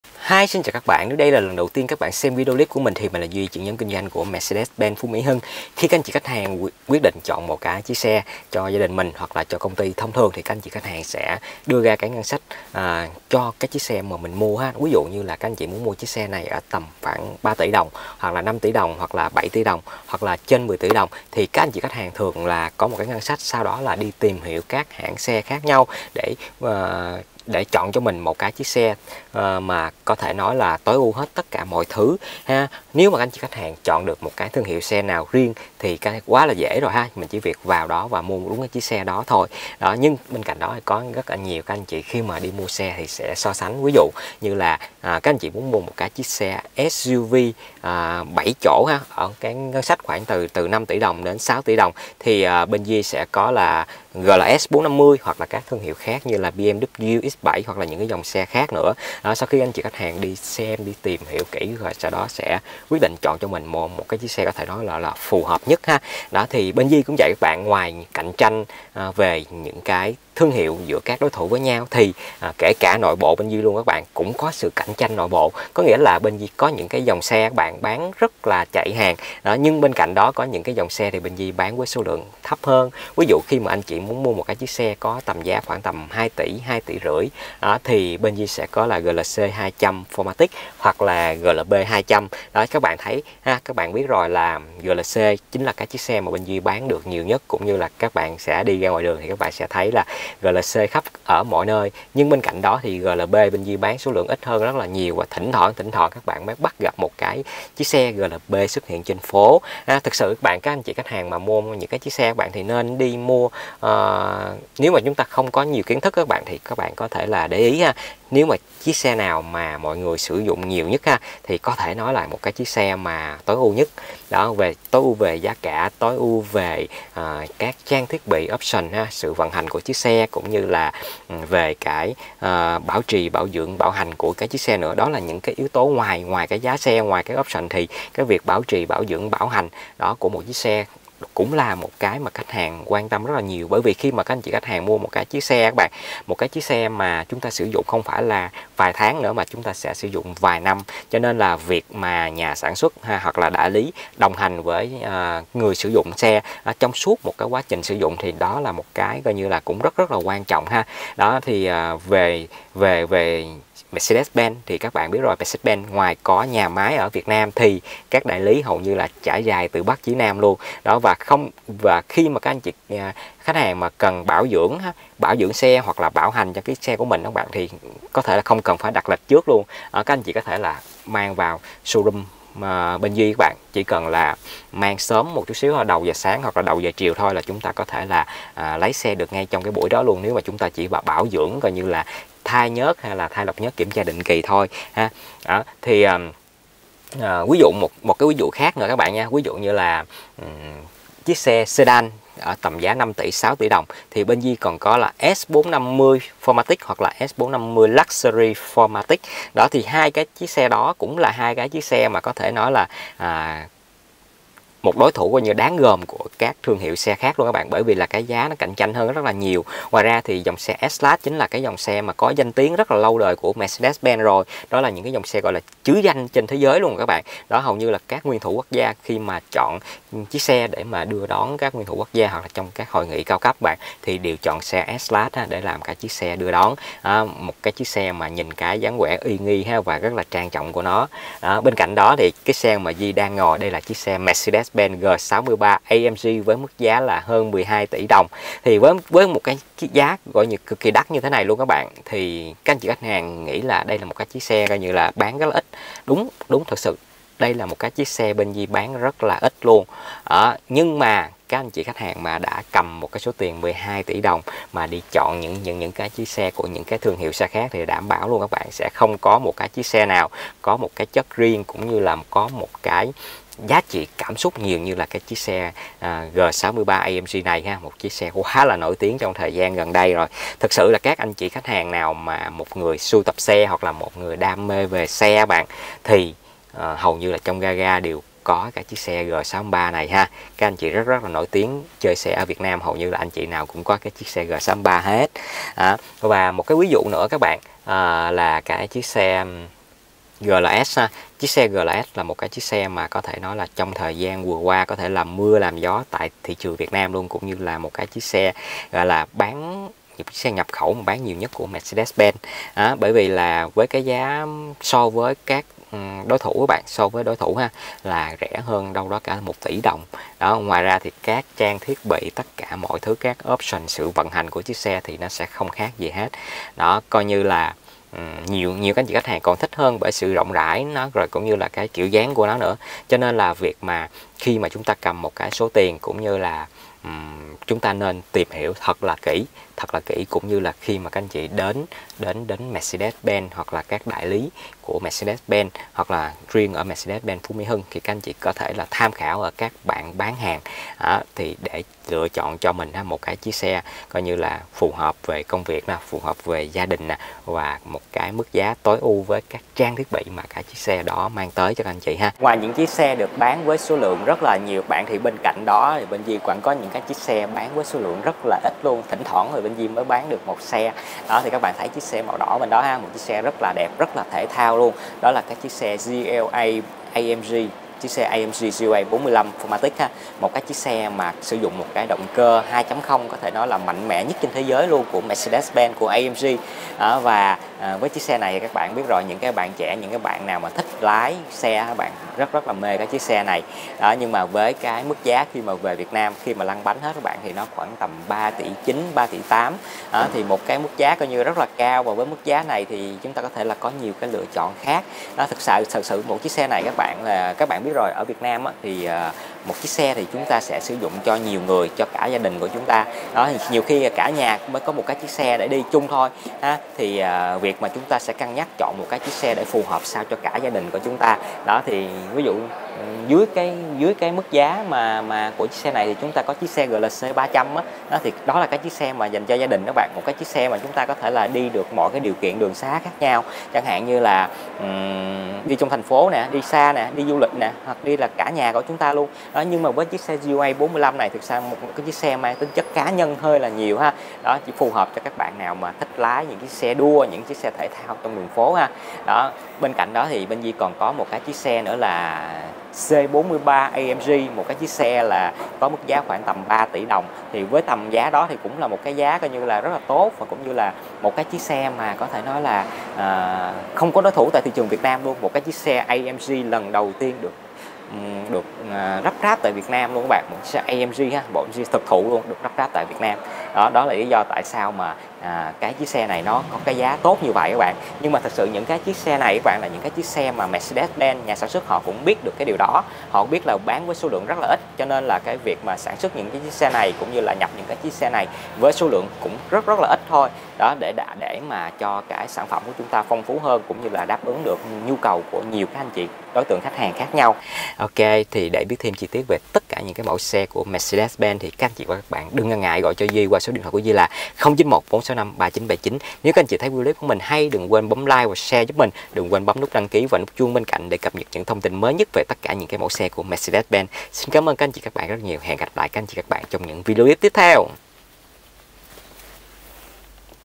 Chọn mua xe ô tô phù hợp với nhu cầu và túi tiền không dễ | Chọn xe Mercedes loại nào thì hợp lí ?
LH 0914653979 ( MR DUY) ĐỂ ĐƯỢC TƯ VẤN CHÍNH XÁC VÀ GIÁ TỐT NHẤT TRONG HỆ THỐNG .811-813 NGUYỄN VĂN LINH QUẬN 7 TP.HCM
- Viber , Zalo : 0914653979